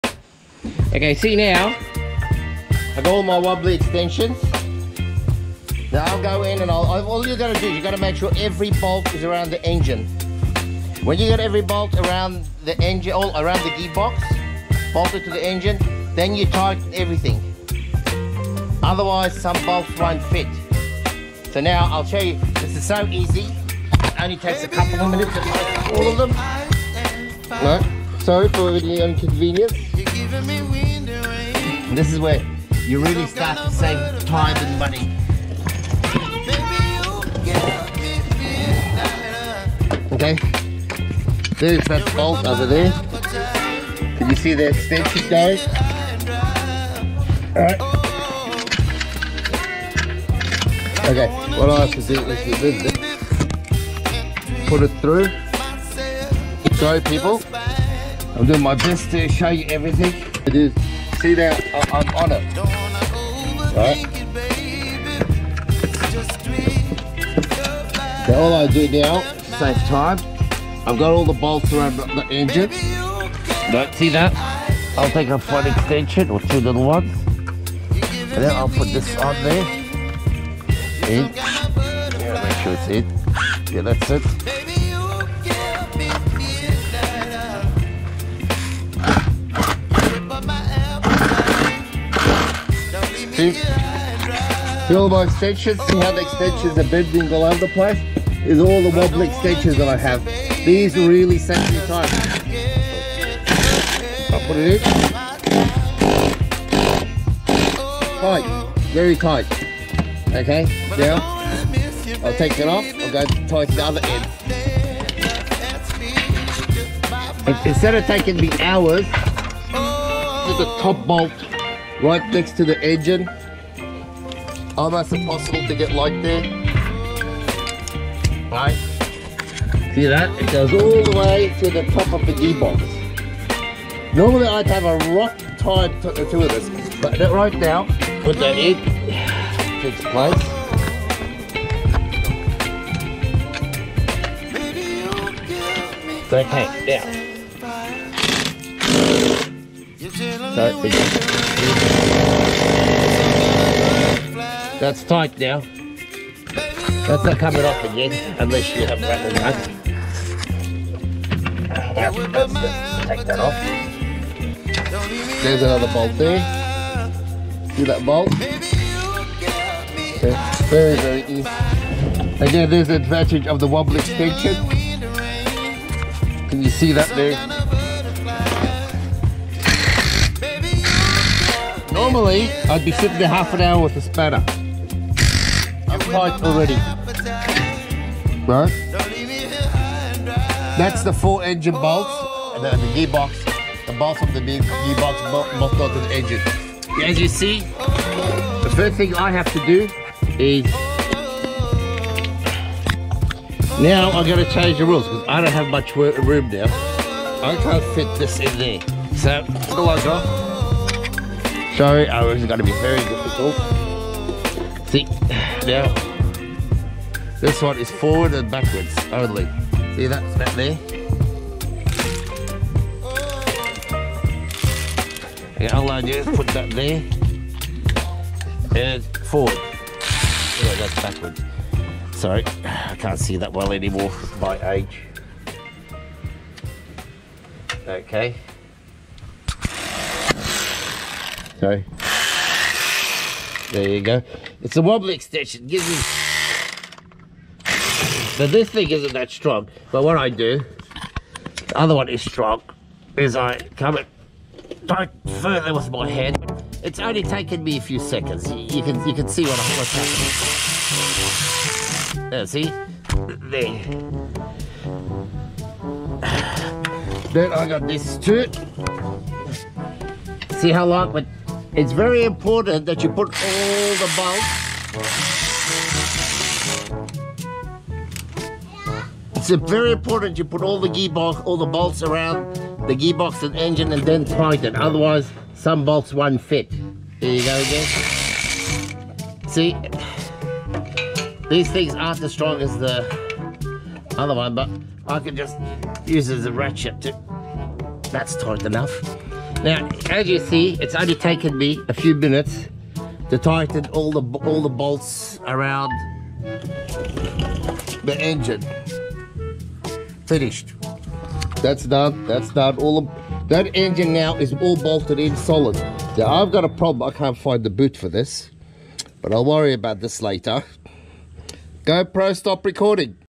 Putting Bolts Around Engine (Part 10)
Getting the right tools really makes the job easy. In todays world. Very few mechanics replace clutches. They make more money from services and roadworthys.